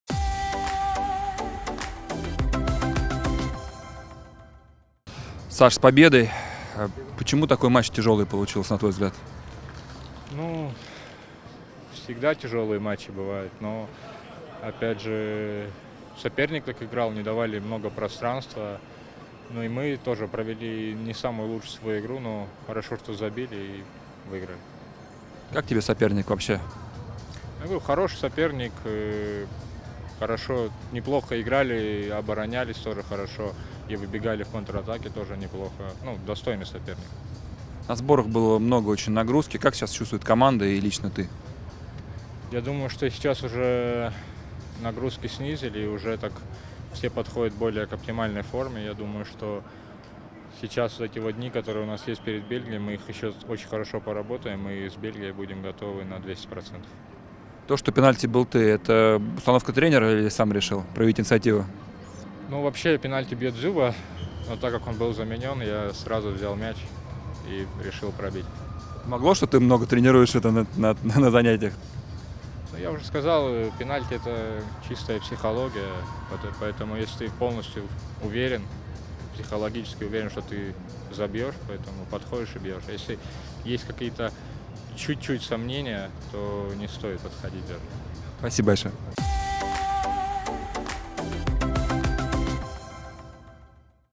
Александр Соболев: "Пенальти - чистая психология"
Автор победного мяча – о забитом пенальти в конце игры.